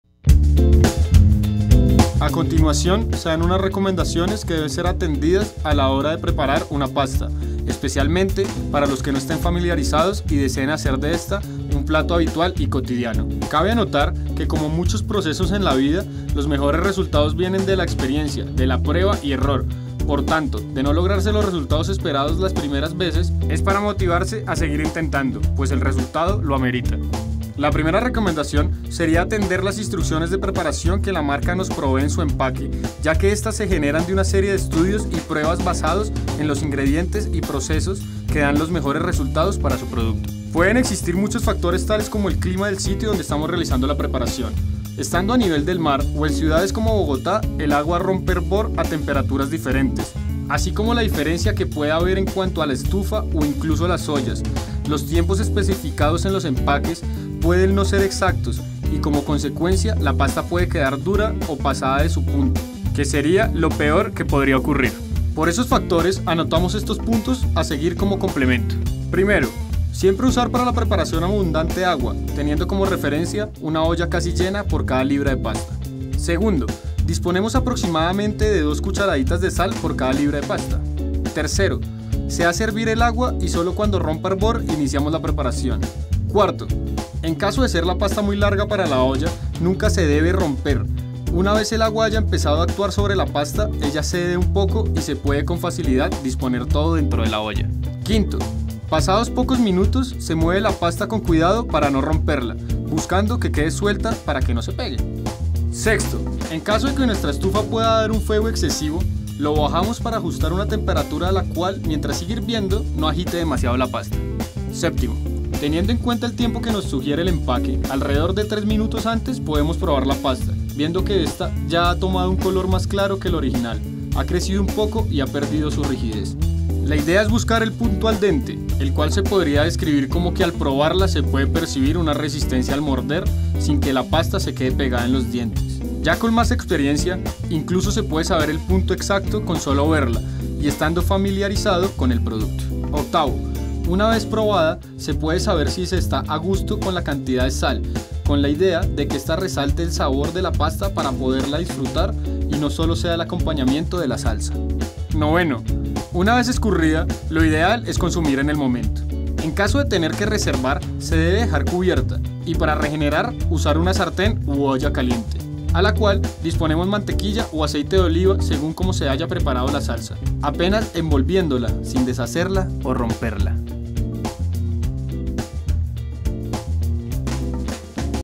Preparación pasta
Ingredientes:

- 2 cucharadas de sal
- 50cc Aceite de Oliva
- 500 grs de Tallarines de Pastas Doria

Preparación:

1. Siempre usar para la preparación abundante agua, teniendo como referencia una olla casi llena por cada libra de pasta.

2. Disponemos aproximadamente de dos cucharaditas de sal por cada libra de pasta.

3. Se hace hervir el agua y solo cuando rompa hervor iniciamos la preparación.

4. En caso de ser la pasta muy larga para la olla, nunca se debe romper, una vez el agua haya empezado a actuar sobre la pasta, esta cederá un poco y se puede con facilidad disponer toda dentro de la olla.

5. Pasados pocos minutos se mueve la pasta con cuidado de no romperla, buscando que quede suelta para que no se pegue.

6. En caso de que nuestra estufa pueda dar un fuego excesivo, bajamos para ajustar una temperatura a la cual siga hirviendo el agua. No agite demasiado la pasta.

7. Teniendo en cuenta el tiempo que nos sugiere el empaque, alrededor de 3 minutos antes podemos probar la pasta, viendo que esta ya ha tomado un color más claro que el original.

8. Ha crecido un poco y  ha perdido su rigidez; La idea es buscar el punto al dente; “al dente” es cuando tomamos uno de los fideos al azar y al partirlo obtenemos un punto blanco en el centro.

9. Una vez probada se puede saber si se está a gusto con la cantidad de sal, con la idea de que esta resalte el sabor de la pasta para poderla disfrutar, y no solo sea el acompañamiento de la salsa.

10. Una vez escurrida, el ideal es consumirse en el momento.

11. En caso de tener que reservar se debe dejar cubierta, y para regenerar usar una sartén u olla caliente con mantequilla o aceite de oliva según como se haya preparado la salsa, apenas envolviéndola sin deshacerla o romperla.